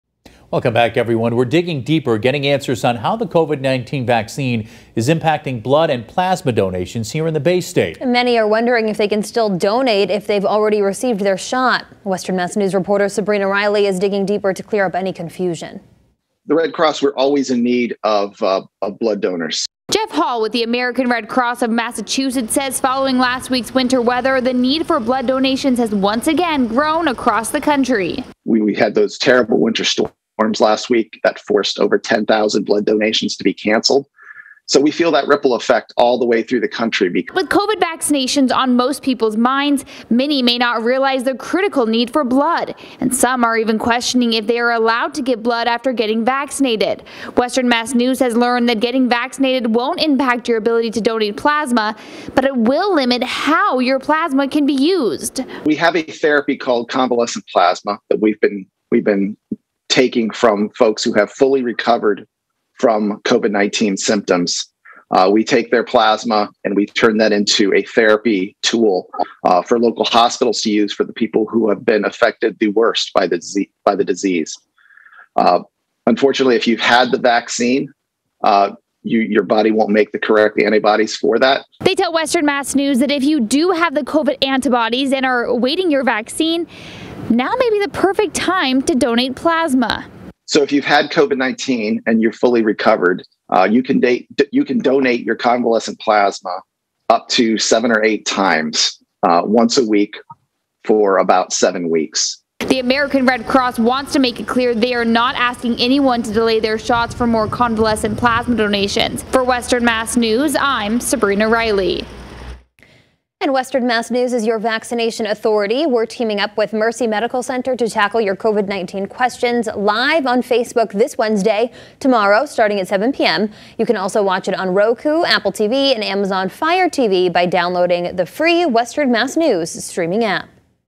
Red Cross discusses COVID-19 vaccine's impact of blood, plasma donations
Western Mass News is Your Vaccine Authority and we are getting answers on how the COVID-19 vaccine is impacting blood and plasma donations in the Bay State.